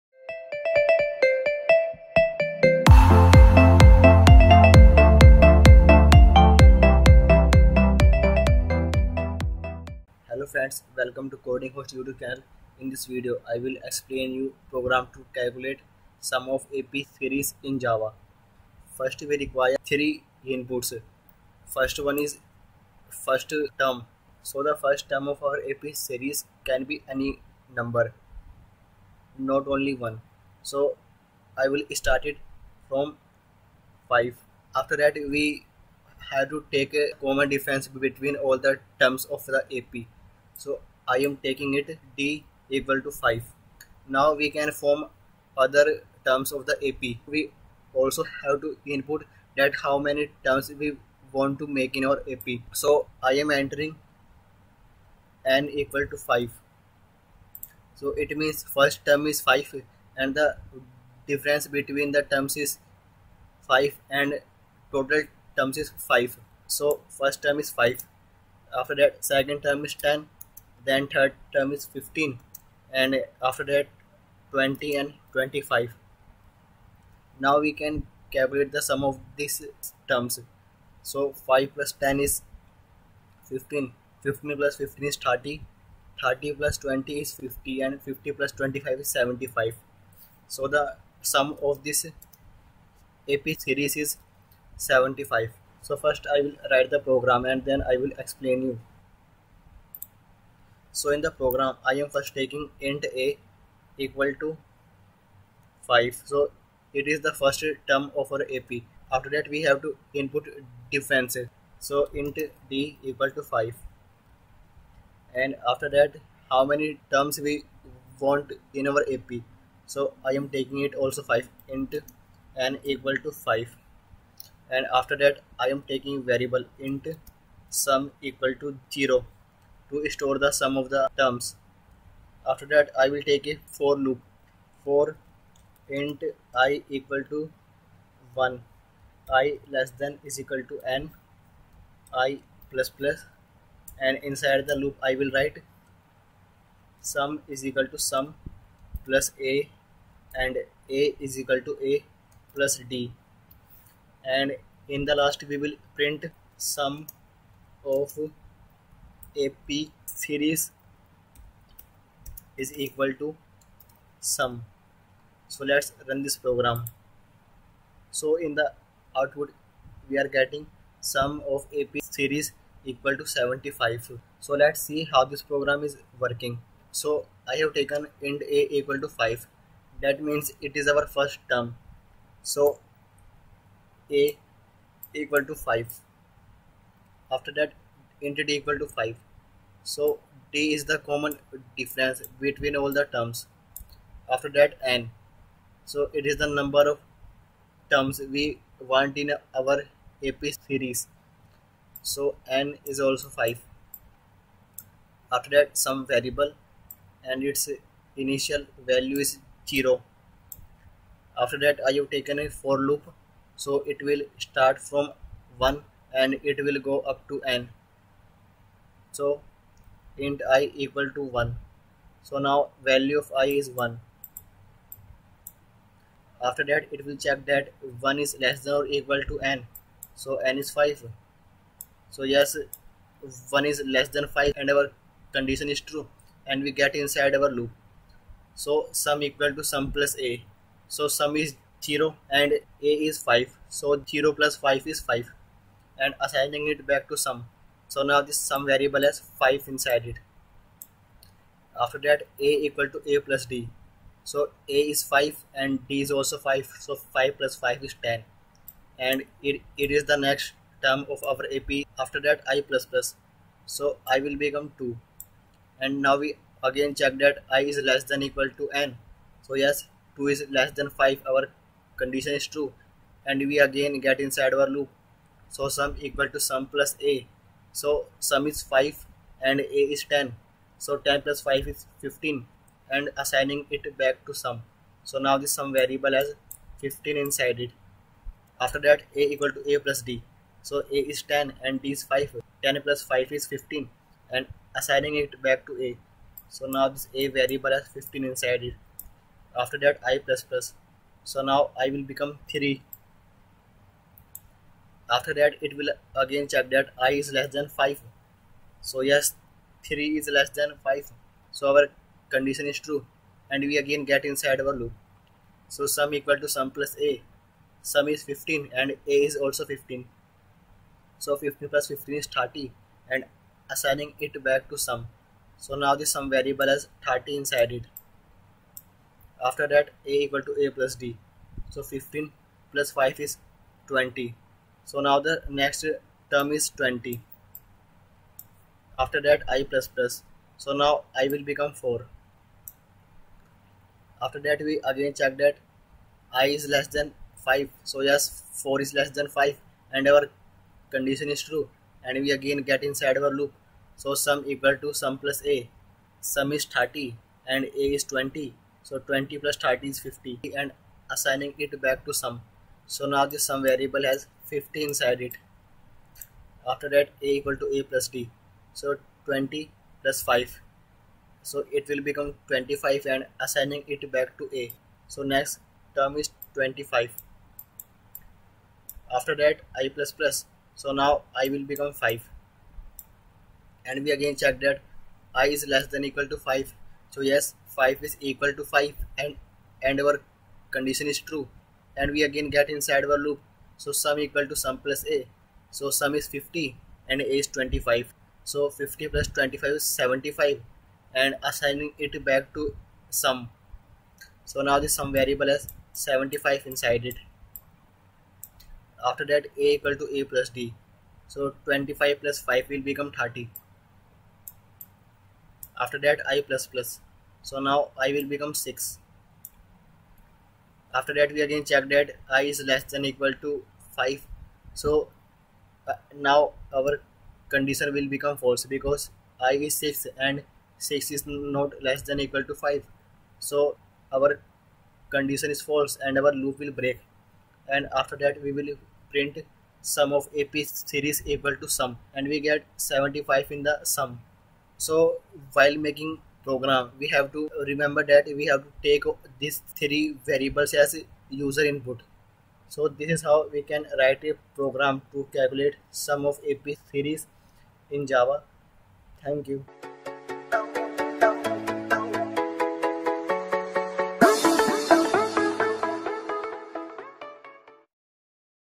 Sum of A. P. series | Sum of A. P. series in java
In this video, I will show you the program to calculate the sum of A.P. series in java.

If you are running this program in online compilers then change the class name to which the class name is already written on that compiler tab.